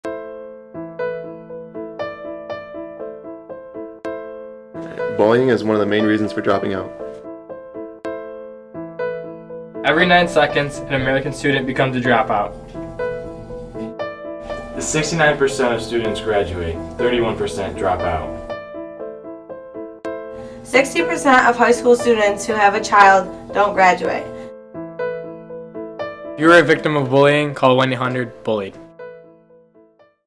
Public Service Announcement: High School Drop Outs
Public Service Announcement made by students in Video Production.

We are high school students, not professionals.